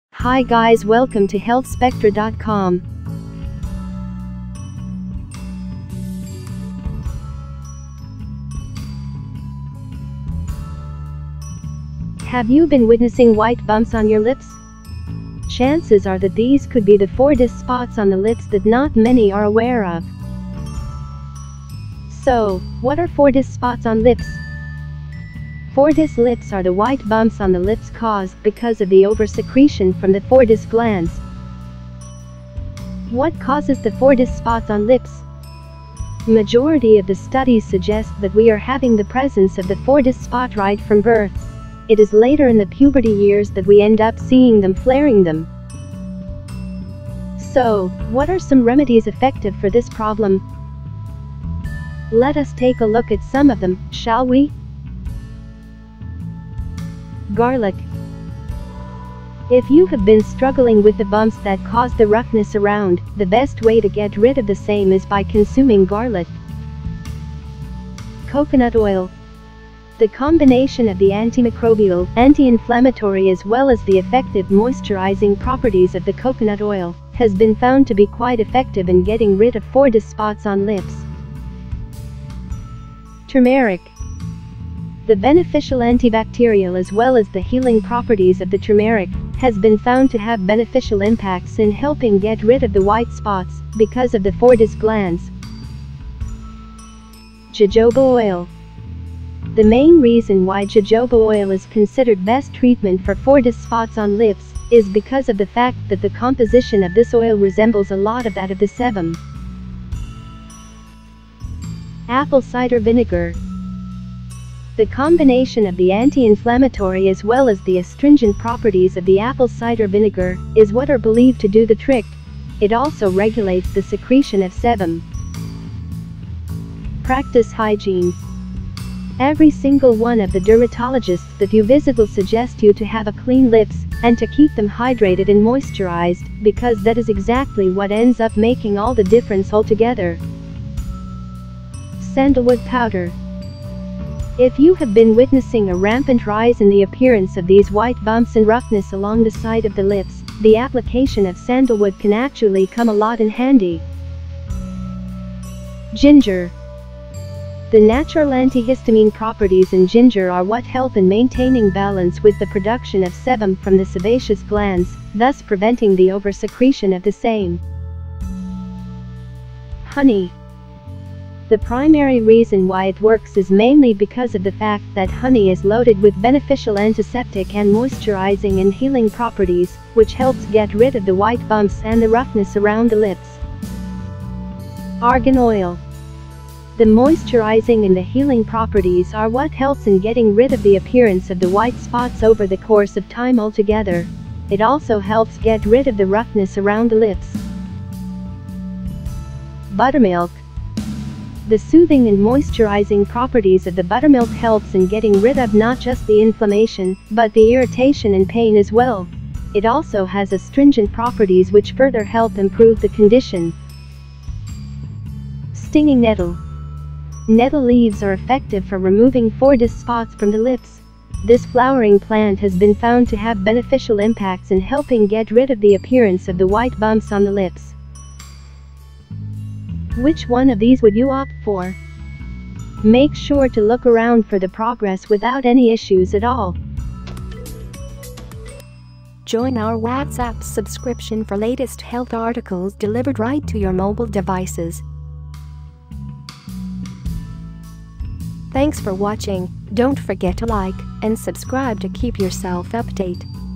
12 Ways To Get Rid Of Fordyce Spots On Lips | Healthspectra
12 Ways To Get Rid Of Fordyce Spots On Lips

Have you been witnessing white bumps on your lips? Chances are that these could be the Fordyce spots on the lips that not many are aware of.

So, what are Fordyce spots on lips? Fordyce lips are the white bumps on the lips caused because of the over secretion from the Fordyce glands.

What causes the Fordyce spots on lips? Majority of the studies suggest that we are having the presence of the Fordyce spot right from birth. It is later in the puberty years that we end up seeing them flaring them.

So, what are some remedies effective for this problem? Let us take a look at some of them, shall we?

1. Garlic. If you have been struggling with the bumps that cause the roughness around, the best way to get rid of the same is by consuming garlic.

2. Coconut Oil. The combination of the antimicrobial, anti-inflammatory as well as the effective moisturizing properties of the coconut oil has been found to be quite effective in getting rid of Fordyce spots on lips.

3. Turmeric. The beneficial antibacterial as well as the healing properties of the turmeric has been found to have beneficial impacts in helping get rid of the white spots because of the Fordyce glands.

4. Jojoba Oil. The main reason why jojoba oil is considered best treatment for Fordyce spots on lips is because of the fact that the composition of this oil resembles a lot of that of the sebum.

5. Apple Cider Vinegar. The combination of the anti-inflammatory as well as the astringent properties of the apple cider vinegar is what are believed to do the trick. It also regulates the secretion of sebum.

6. Practice hygiene. Every single one of the dermatologists that you visit will suggest you to have a clean lips and to keep them hydrated and moisturized because that is exactly what ends up making all the difference altogether.

7. Sandalwood Powder. If you have been witnessing a rampant rise in the appearance of these white bumps and roughness along the side of the lips, the application of sandalwood can actually come a lot in handy.

8. Ginger. The natural antihistamine properties in ginger are what help in maintaining balance with the production of sebum from the sebaceous glands, thus preventing the over secretion of the same.

9. Honey. The primary reason why it works is mainly because of the fact that honey is loaded with beneficial antiseptic and moisturizing and healing properties which helps get rid of the white bumps and the roughness around the lips.

10. Argan Oil. The moisturizing and the healing properties are what helps in getting rid of the appearance of the white spots over the course of time altogether. It also helps get rid of the roughness around the lips.

11. Buttermilk. The soothing and moisturizing properties of the buttermilk helps in getting rid of not just the inflammation but the irritation and pain as well. It also has astringent properties which further help improve the condition.

12. Stinging nettle. Nettle leaves are effective for removing Fordyce spots from the lips. This flowering plant has been found to have beneficial impacts in helping get rid of the appearance of the white bumps on the lips.

Which one of these would you opt for? Make sure to look around for the progress without any issues at all.